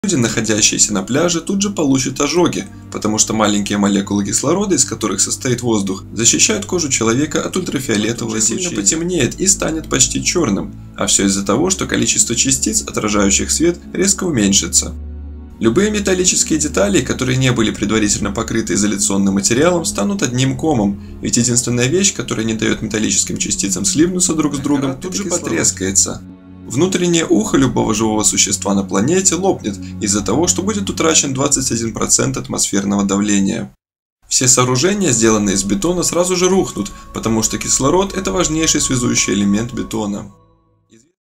Что будет если Земля останется без кислорода на 5 секунд?
Кислород главная составляющая всей жизни на Земле!Если кислород исчезнет это мгновенно приведет к катастрофическим последствиям.